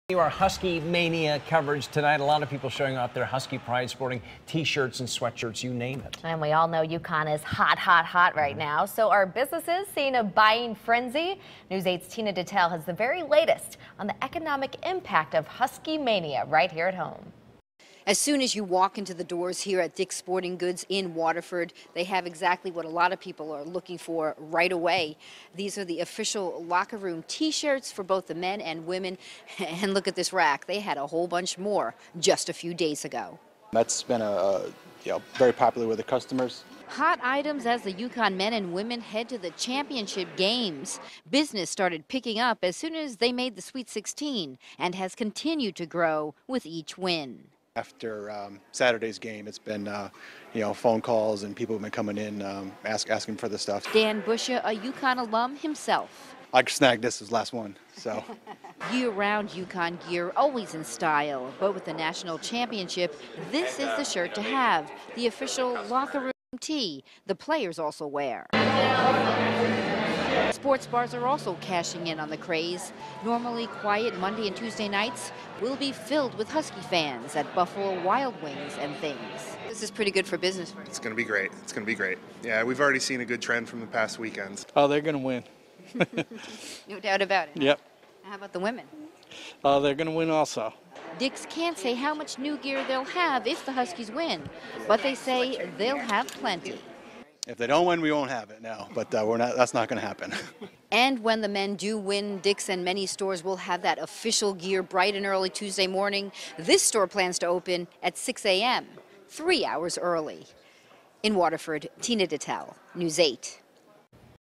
Huskie's march to championship good for local business
A lot of people showing off Husky pride buying t-shirts, sweatshirts, you name it. So how much does local business benefit from the huskies march to the championship?